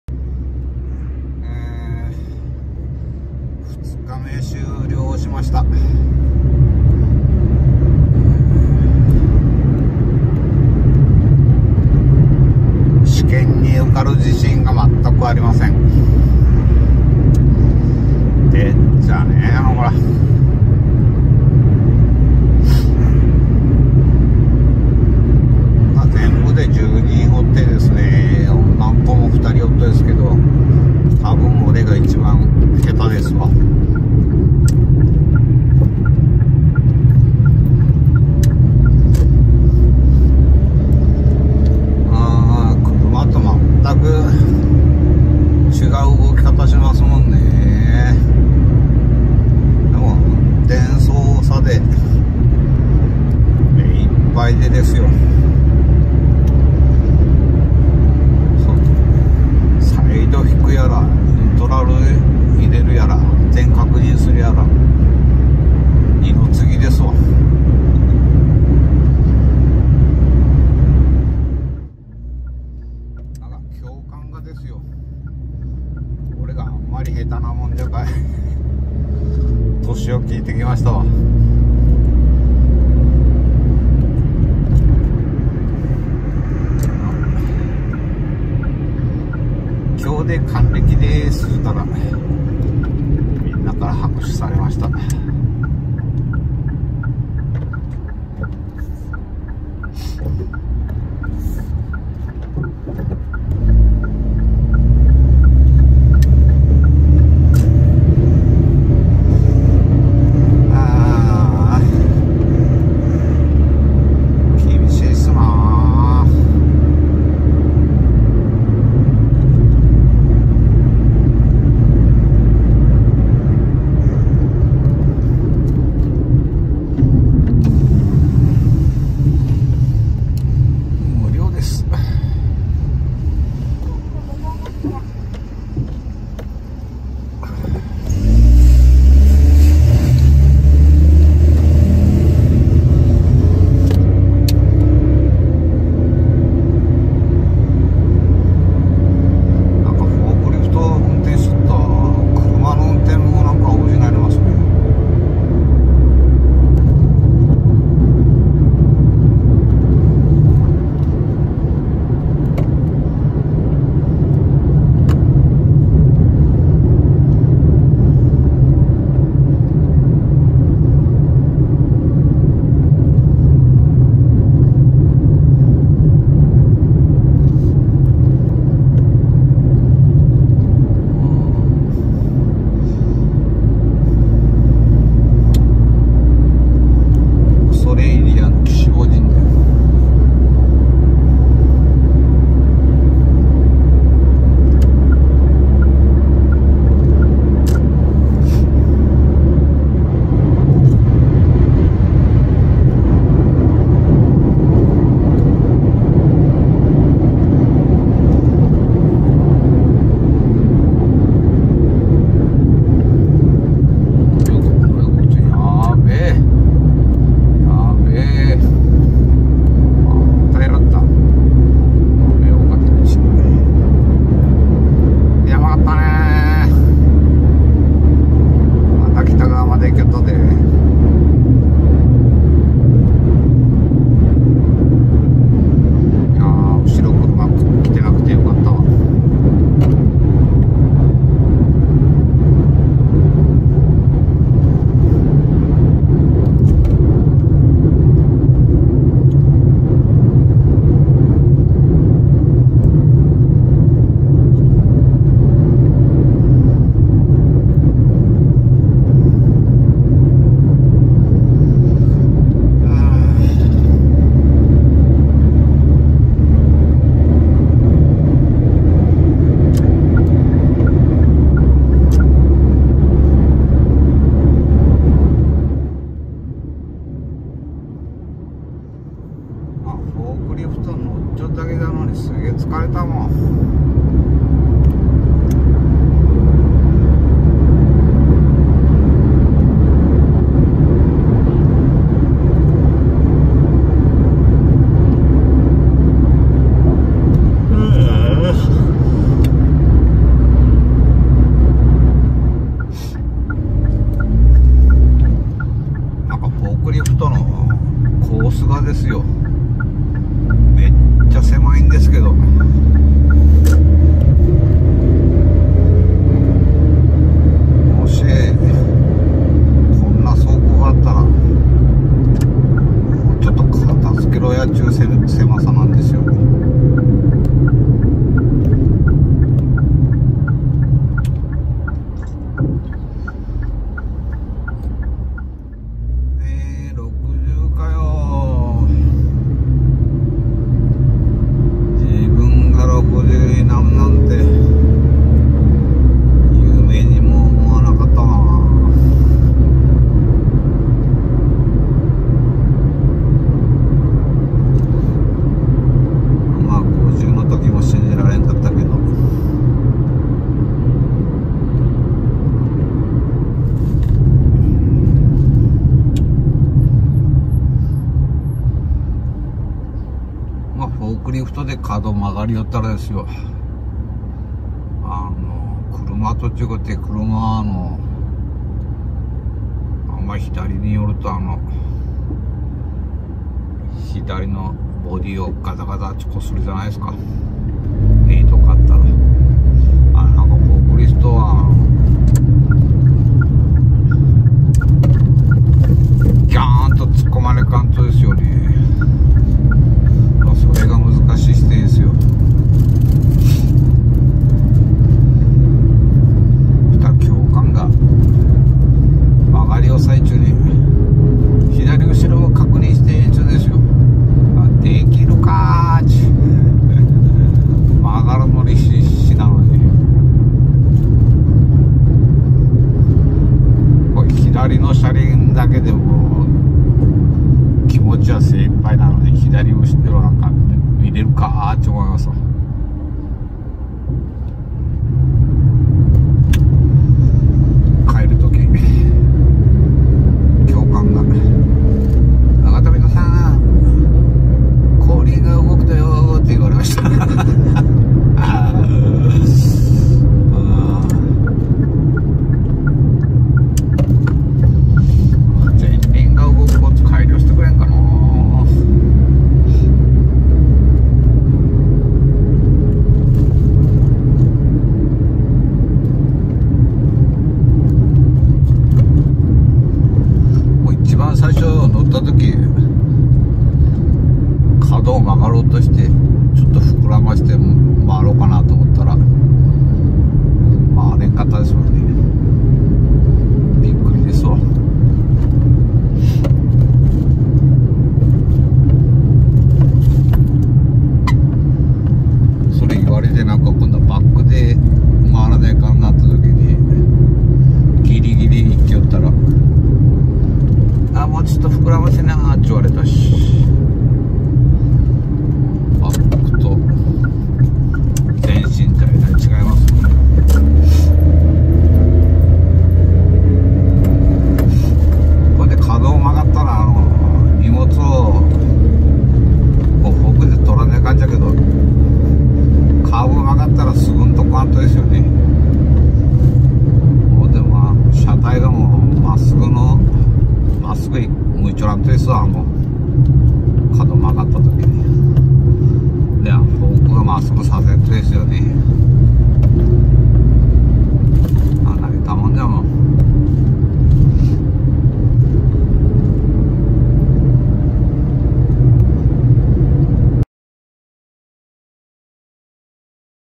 2022 01 19 初めてのフォークリフト実技
国が2割負担してくれるという甘い言葉に乗せられて、とりに行ったものの
ムズ過ぎて試験に通る気がしないｗ